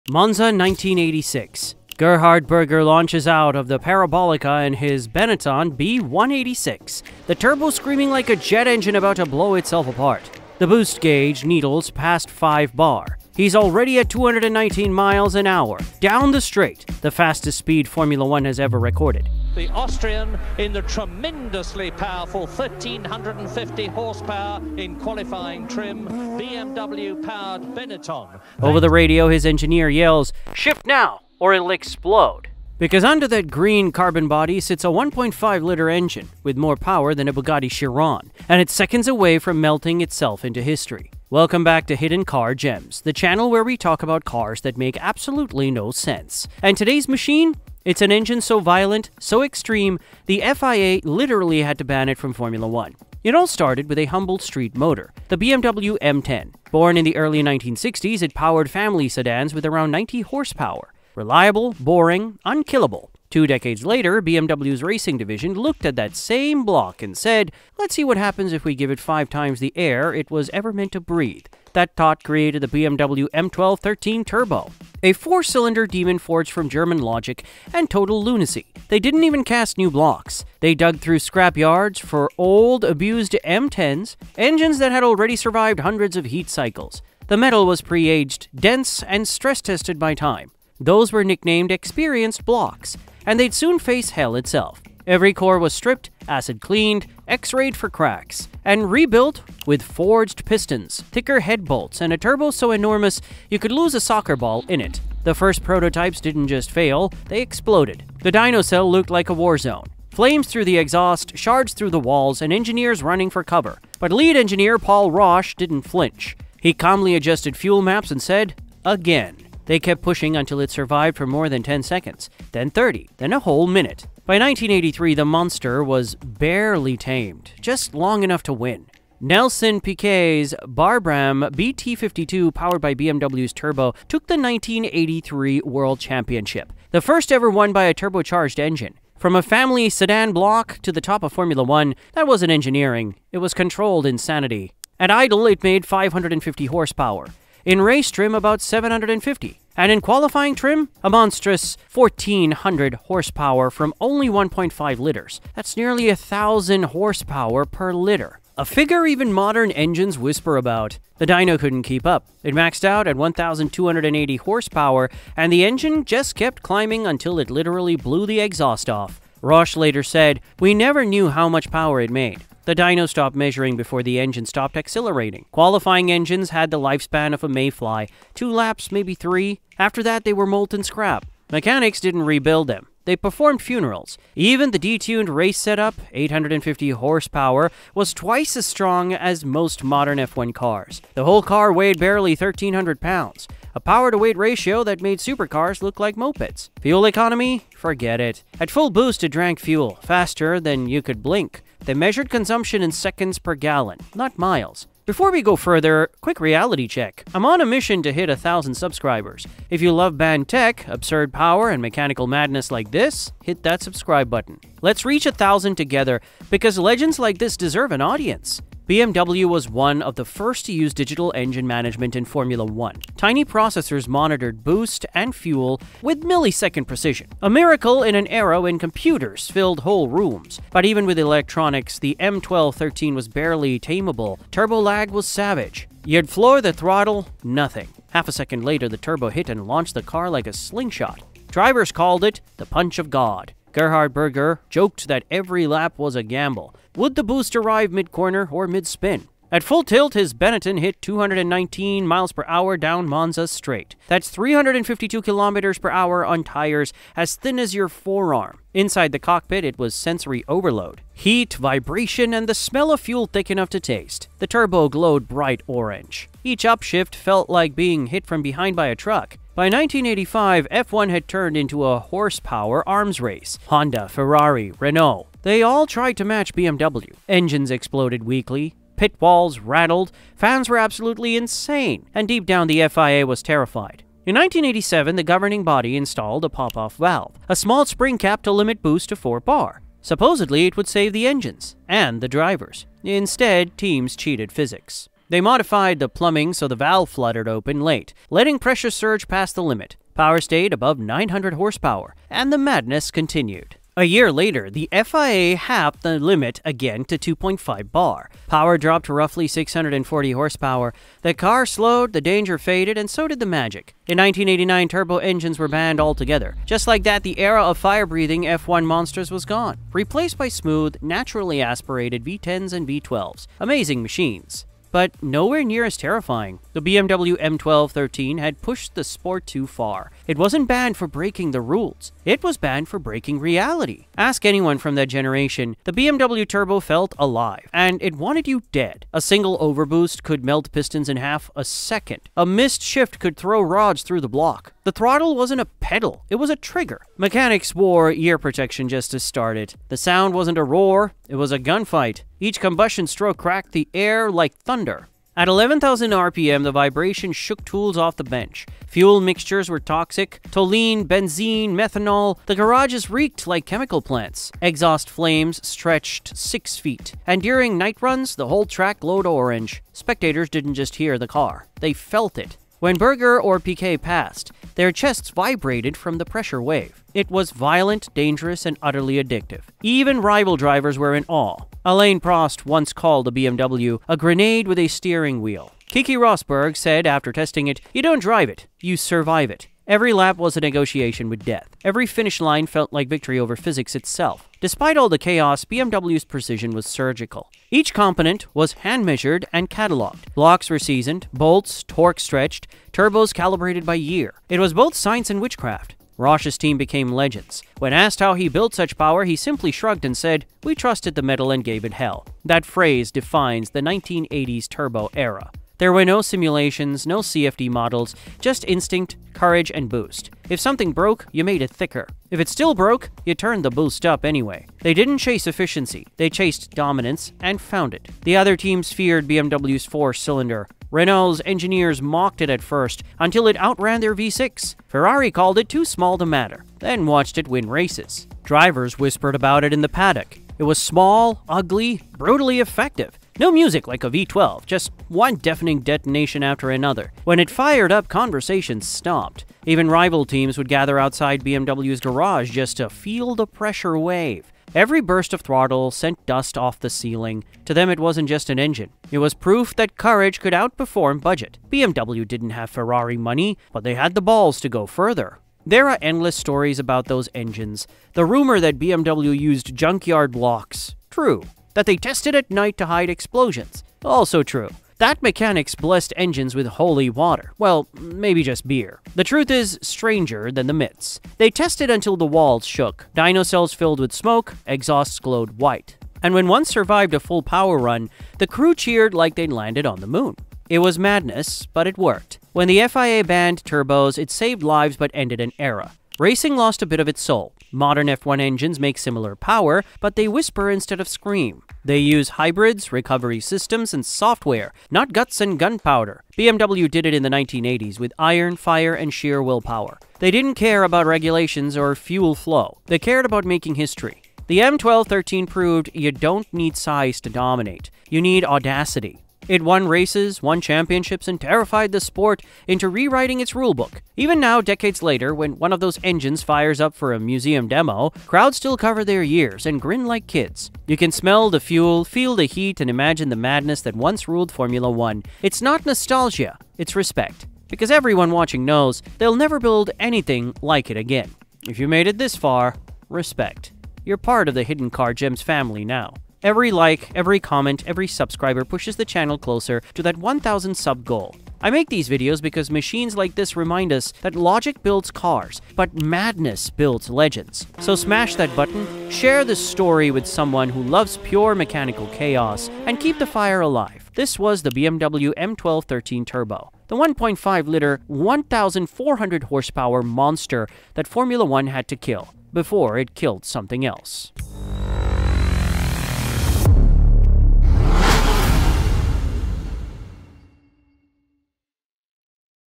The Engine That BROKE Formula 1 - BMW's 1.5 Turbo
BMW built an engine so powerful it broke Formula 1.
A tiny 1.5-liter turbo that made 1 400 horsepower — from scrap-yard blocks.
It turned underdogs into world champions… until the FIA had to ban it.

Welcome back to Hidden Car Gems, the channel where we talk about cars that make absolutely no sense.
In this episode, we dive into the BMW M12/13 Turbo — the most insane power unit ever to hit Formula 1.
Born from the humble BMW M10 street engine, it became a 1.5-liter bomb that changed racing forever.

👉 Stay till the end to see how BMW engineers used junk engines, brute boost, and pure madness to dominate — and why Formula 1 said “never again.”